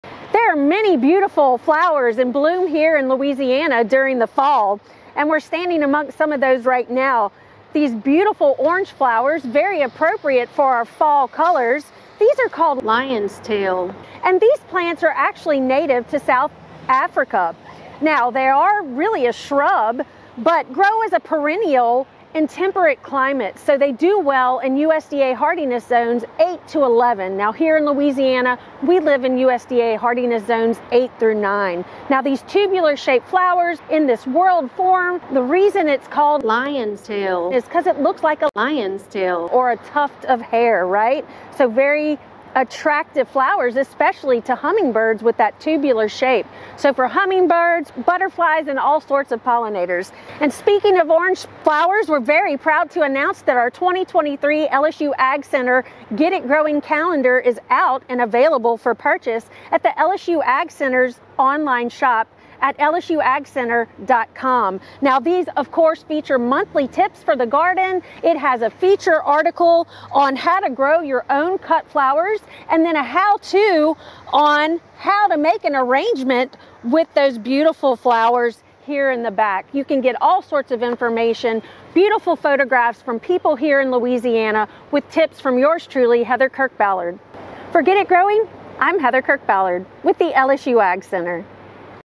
Get It Growing: Lion’s tail has bright orange flowers that attract butterflies
Lion’s tail is a plant that provides beautiful, colorful flowers to the fall landscape. On this edition of Get It Growing, LSU AgCenter horticulturist Heather Kirk-Ballard explains how lion’s tail is also a great plant to attract hummingbirds and butterflies.